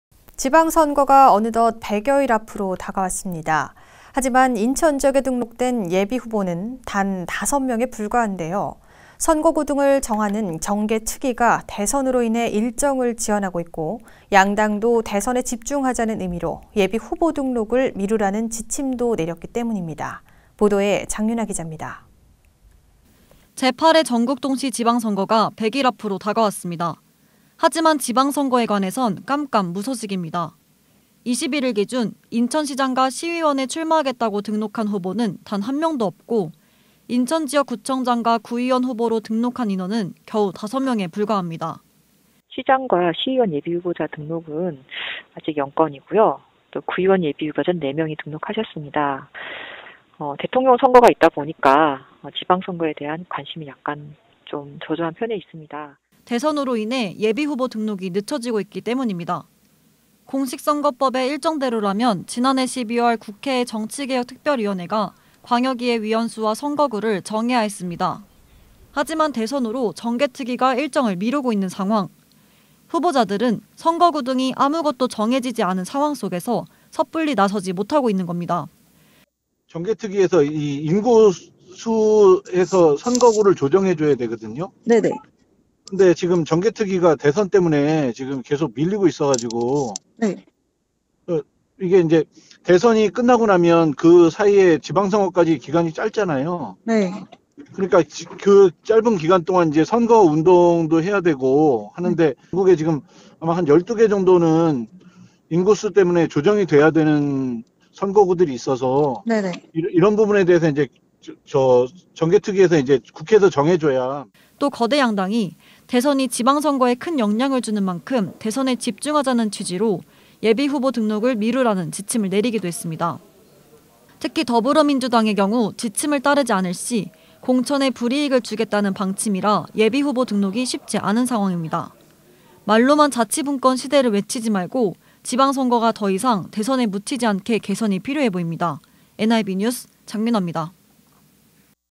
[NIB 뉴스] 지방선거일 100일 앞으로… 등록된 예비후보자는 5명에 불과
앵커멘트)
지방선거가 어느 덧 100여 일 앞으로 다가왔습니다. 하지만, 인천지역에 등록된 예비후보는 단 5명에 불과합니다. 선거 구 등을 정하는 정개특위가 대선으로 인해 일정을 지연하고 있고, 양당도 대선에 집중하자는 의미로 예비후보 등록을 미루라는 지침도 내렸기 때문인데요. 보도에 장윤아 기자입니다.

리포트)
제8회 전국동시지방선거가 100일 앞으로 다가왔습니다.

하지만, 지방선거에 관해선 깜깜 무소식입니다.

21일 기준, 인천시장과 시의원에 출마하겠다고 등록한 후보는 단 한 명도 없고,

인천지역 구청장과 구의원 후보로 등록한 인원은 겨우 5명에 불과합니다.

인터뷰) 박선영 / 인천시선거관리위원회 홍보과 공보계장
“시장과 시의원 예비 후보자 등록은 0건이고요. 구의원 예비후보등록자는 4명이 등록하셨습니다. 대통령 선거가 있다 보니까 지방선거에 대한 관심이 약간 저조한 편입니다.”

대선으로 인해 예비 후보등록이 늦춰지고 있기 때문입니다.

공식선거법의 일정대로라면 지난해 12월, 국회의 정치개혁특별위원회가 광역의회 의원 수와 선거구를 정해야했습니다.

하지만, 대선으로 정개특위가 일정을 미루고 있는 상황.

후보자들은 선거구 등이 아무것도 정해지지 않은 상황 속에서 섣불리 나서지 못하고 있는 겁니다.

인터뷰) 신희섭 / 한국지정학연구원 원장
“정개특위에서 인구 수와 선거구를 조정해줘야 하거든요. 그런데 정개특위가 대선 때문에 계속 밀리고 있어가지고 대선이 끝나고 나면 지방선거까지 기간이 짧잖아요. 그 짧은 기간에 선거운동도 하고 해야 하는데, 전국에 12개 정도는 인구 수 때문에 조정이 돼야하는 선거구들이 있어서 이런 부분에 있어서 정개특위 국회가 정해줘야 되는데… ”

또, 거대 양당이 대선이 지방선거에 큰 영향을 주는 만큼 대선에 집중하는 취지로 예비후보 등록을 미루라는 지침을 내리기도 했습니다.

특히, 더불어민주당의 경우 지침을 따르지 않을 시, 공천에 불이익을 주겠다는 방침이라 예비후보 등록이 쉽지 않은 상황입니다.

말로만 자치분권 시대를 외치지 말고,
지방선거가 더 이상 대선에 묻히지 않게 개선이 필요해 보입니다.

nib뉴스 장윤아입니다.

영상취재 김근우 기자